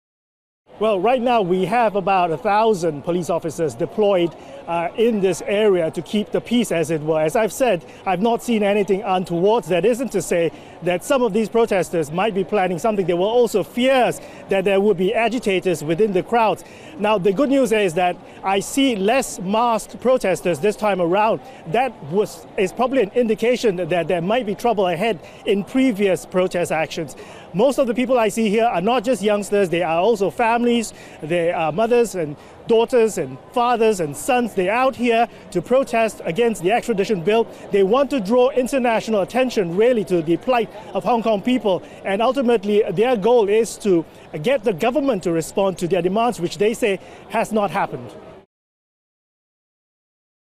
Hong Kong protesters march from Tsim Sha Tsui to West Kowloon station
Hong Kong protesters march from Tsim Sha Tsui to West Kowloon train station, the first large-scale demonstration since Monday's storming of the Legislative Council building. CNA's Roland Lim with the latest.